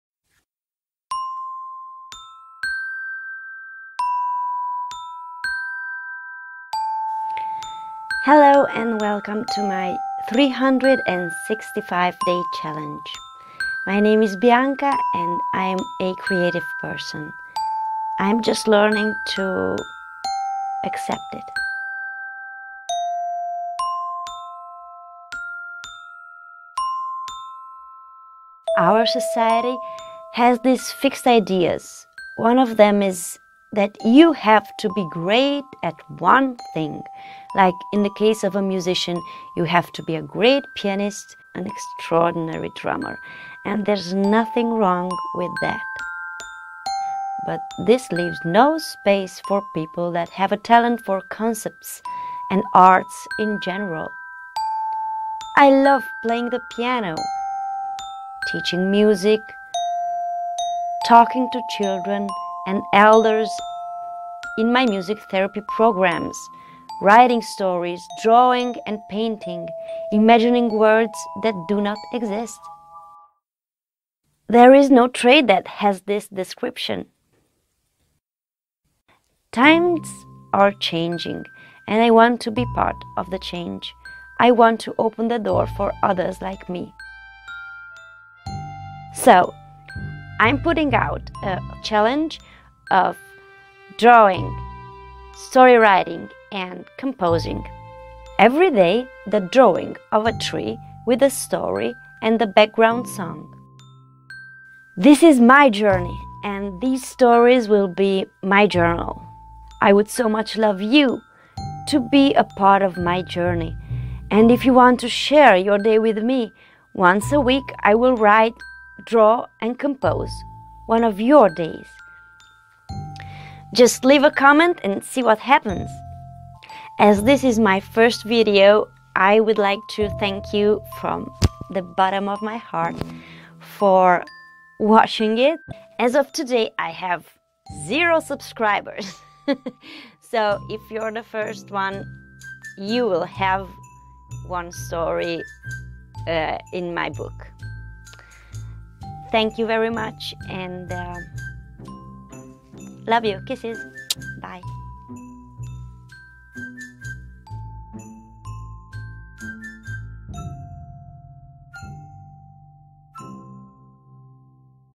I am starting a 366 days creative challenge! Painting, composing, writing for self-improvement
Hello, all you Stardust people out-there !!
Welcome to my 366 Day Creative  Challenge.
Now. this is an art story about how a childhood weakness gets turned into a strength with the help of uncommonly generous people. I hope this will be for me as much as for you a journey of self-discipline motivation and self-development lesson.
 I don't know about you, but I was raised with this concept that.. you have to have a trade, have to specialize in something in order to be part of society.
My identity was crushed when at 10, being a student in a very competition-driven music school, coming from a family of musicians, I heard my favorite teacher describing me as

​,, good at arts, in general, but not in my musical instrument in special ".

​These words are hunting me ever since ... and made me not even try to be great at anything... just got on with my life as it came. The good thing is that I have never stopped dreaming, drawing, composing, writing lyrics and stories because - and I hope someone out there understands -  it's a stronger than life urge that never stops.
​Now I see a change in the world and I am hoping to be part of this change! I come to the understanding that  If  I choose to, I can change the world, beginning with the way that I see it.  I choose to live in a world where ,,Creatives'' are just as important as any other trade,  and in developing my gifts I can bring value to the world.
And if this can help just one person that has my same difficulty find peace and a  place in this world, I am happy!
​I will give all my energy, time, and heart to see this project fulfilled because I believe very strongly in the fact that everything we see now, was once imagined. And I imagine a bright future for Creatives like me.
Here's to self-development, self-improvement, self-discipline!
 I will have to make lots of changes, but I am sure this will be an awesome inspiration, even if for just some of you!

If you are curious about anything regarding my videos, please live a message in the comments below.

And if you want to help to level up my videos,